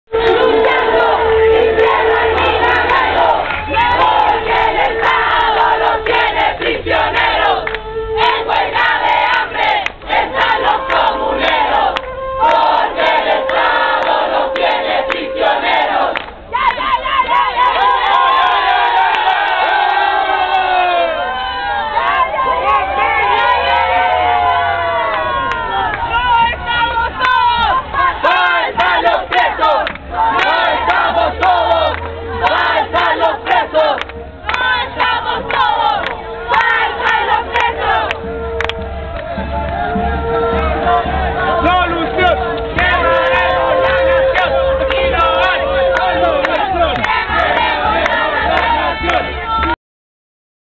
MARCHA EN APOYO A PRESOS POLITICOS MAPUCHE EN HUELGA DE HAMBRE
En la afueras de la carcel de temuco donde se encuentran 14 comuneros mapuches en huelga de hambre extrema de un total de 32 a nivel nacional , marchan en repudio a contra el estado chileno que los encárcela .
los manifestantes repudian los juicios y montajes en contra de los comuneros mapuches y sus respectivas comunidades o lugares donde viven .